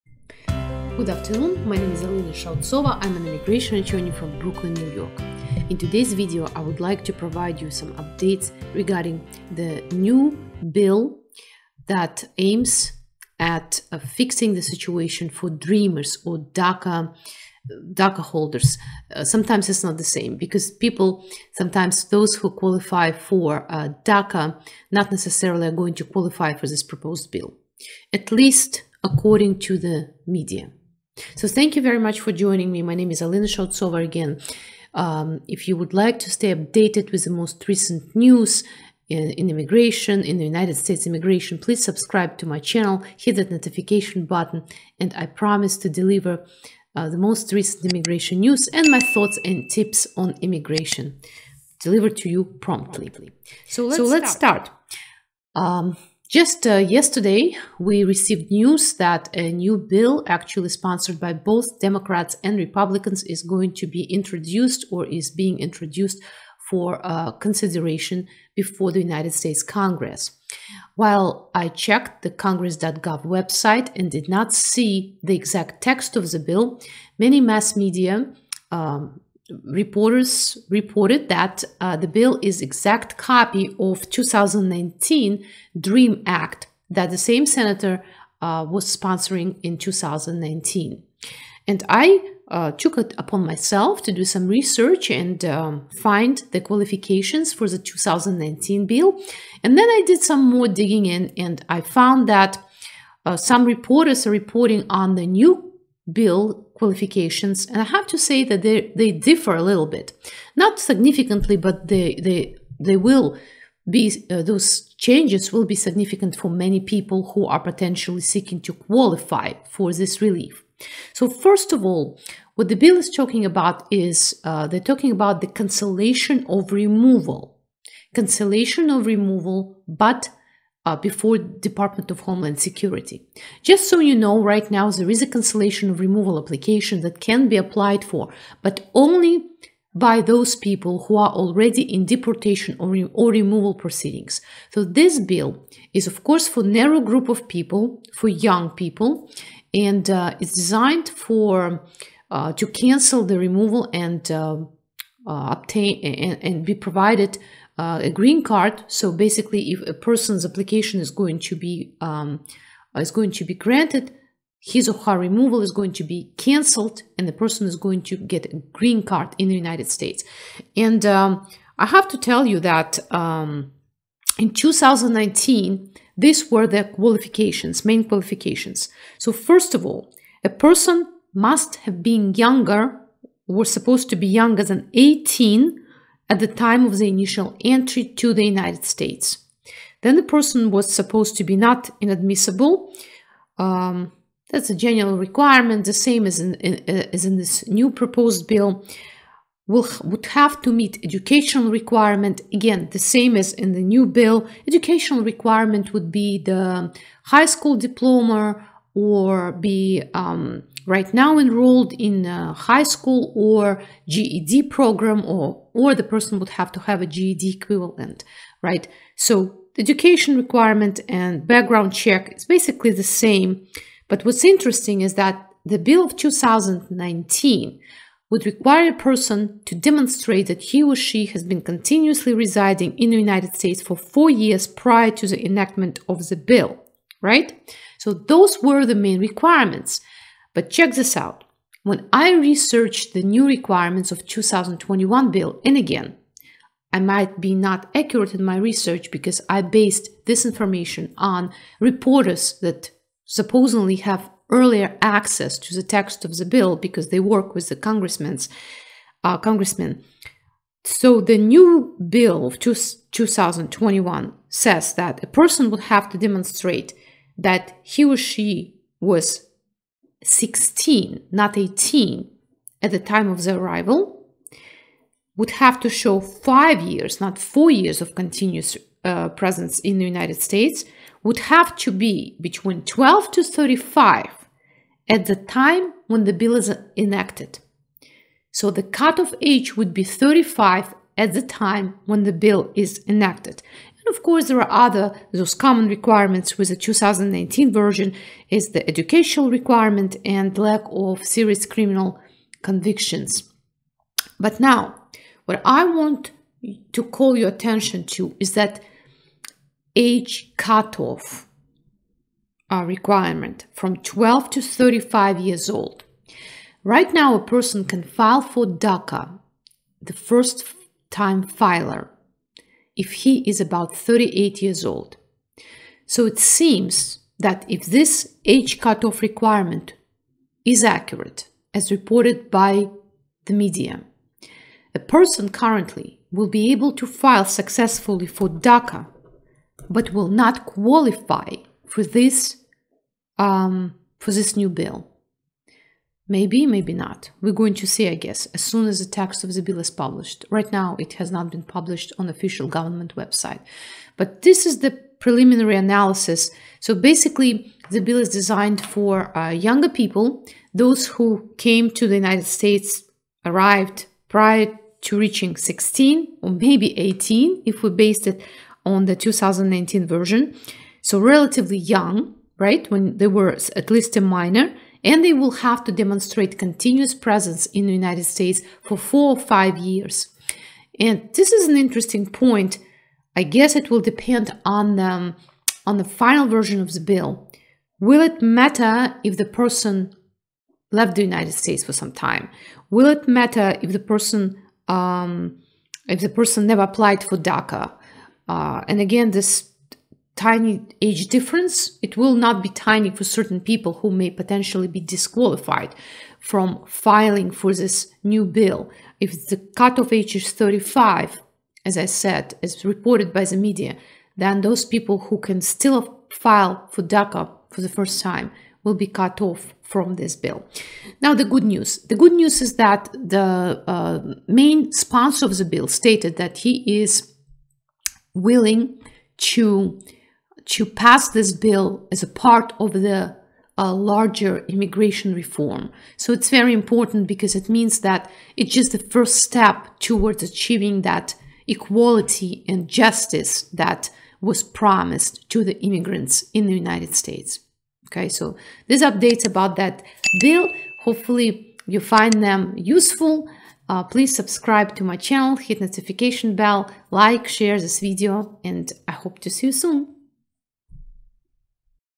Latest Immigration News Dream Act 2021 Updates: Bipartisan Immigration Reform Bill | USA Immigration
Alena Shautsova,  NYC Immigration lawyer, provides updates regarding Bipartisan Immigration Bill Dream Act 2021and compares it to the Dream Act 2019 introduced by Sen. Graham.